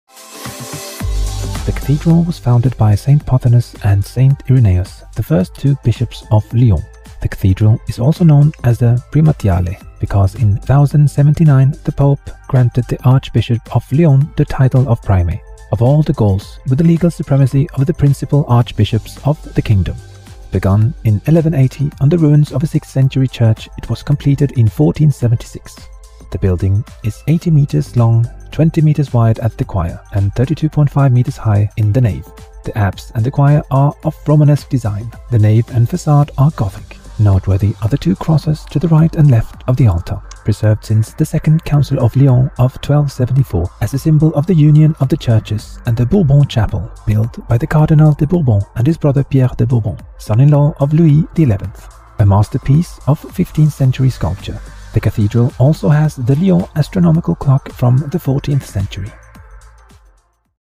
LYON CATHEDRAL - FRANCE, LYON
Address: Place Saint-Jean, 69005 Lyon, France
Opened: 1480
Architect: Jacques de Beaujeu
Architectural styles: Gothic architecture, Romanesque architecture
Burials: Joseph-Alfred Foulon, Pierre-Hector Coullié

Local Traveller
By Willo’s Worlds